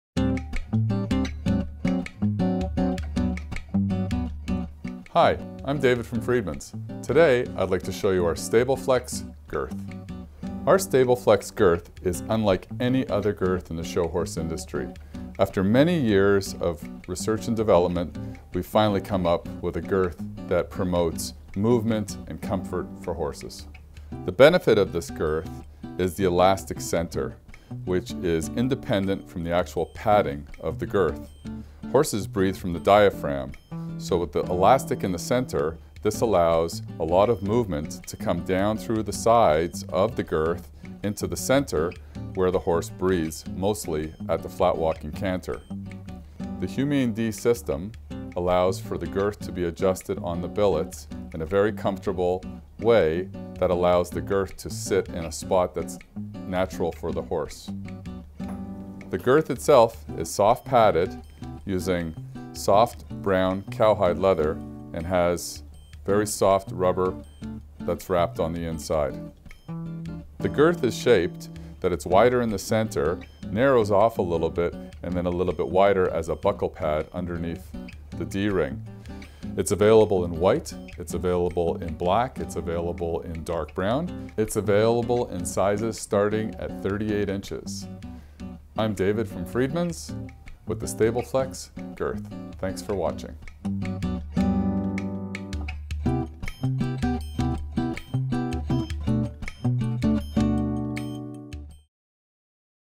Stabil-Flex Girth
Stability, flexibility and comfort is exactly what this amazing girth offers.

With five years of development and testing this is the best humane Dee girth on the market. The girth flexes (stretches) on an independent elastic system that is positioned in the center of the girth. This allows the girth to flex from where the horse breathes. Combined with an anatomical shape and a soft cow hide padding this humane girth offers performance enhancing properties that will be a positive addition to your everyday training or show equipment.

The Stabil-Flex Girth also features 2 center Dee rings to conveniently attach a martingale or brace cord. All buckles and Dees are stainless steel.